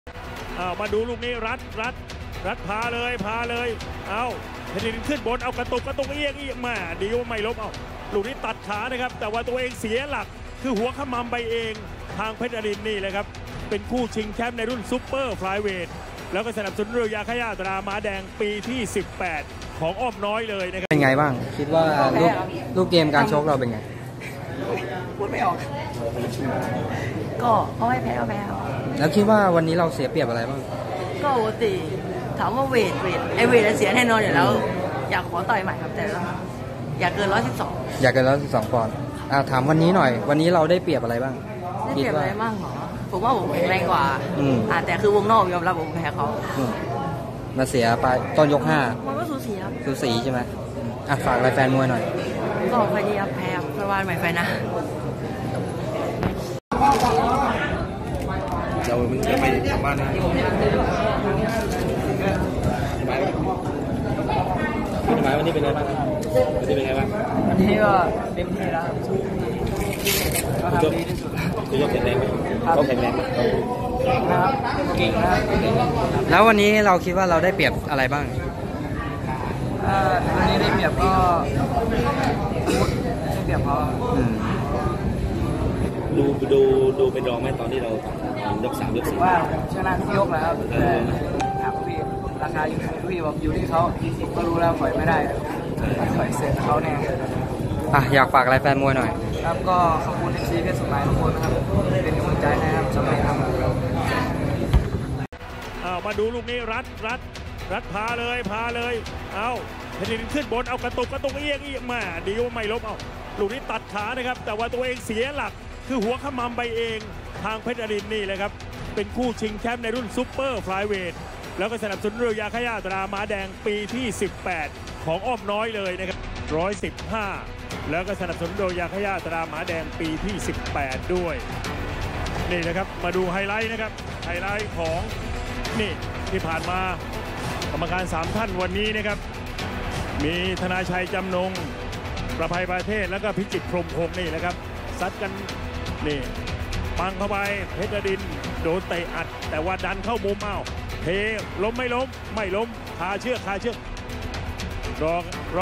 เพชรสมหมายว่าชนะทุกยกวันนี้แต่ลูกพี่บอกราคาเป็นรอง
ชมเบื้องหลัง 🔴เพชรสมหมาย ส.สมหมาย ชนะคะแนน 🔵เพชรนรินทร์ จิตรเมืองนนท์